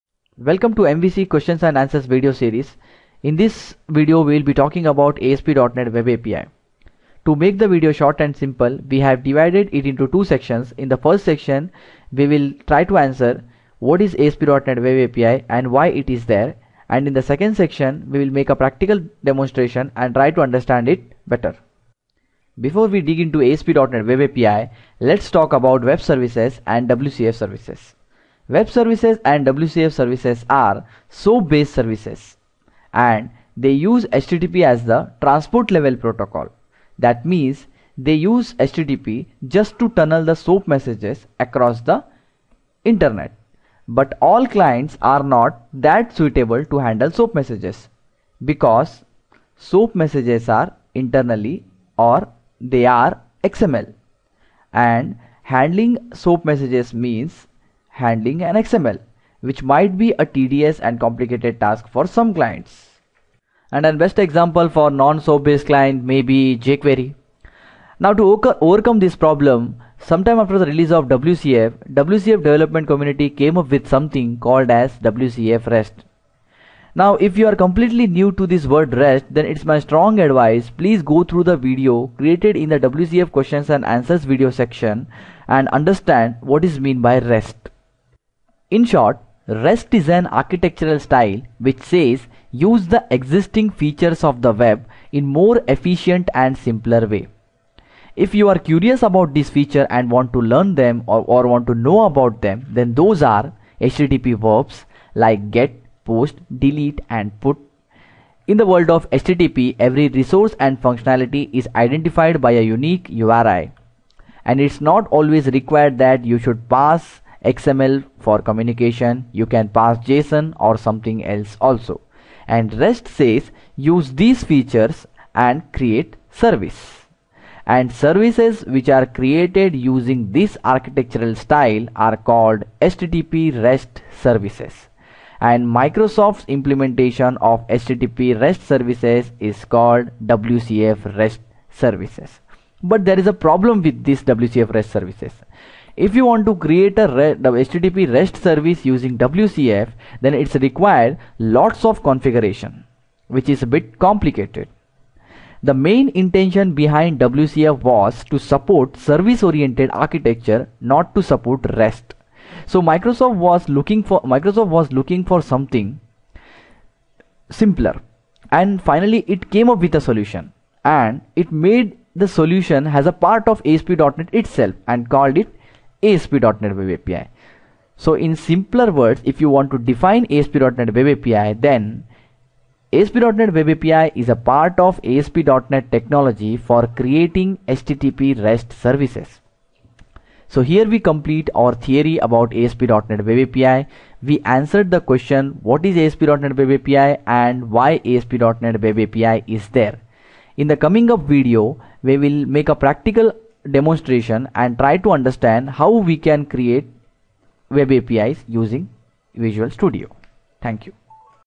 MVC - Asp.net Web API Part 1( Theory )
n this video we will try to understand what is Web API. This Video is divided in to two parts first one runs through theory and the next video following this will demonstrate how to implement Web API using MVC ( Model view controller).
This is also a awesome MVC interview question which is making round in ASP.NET interviews.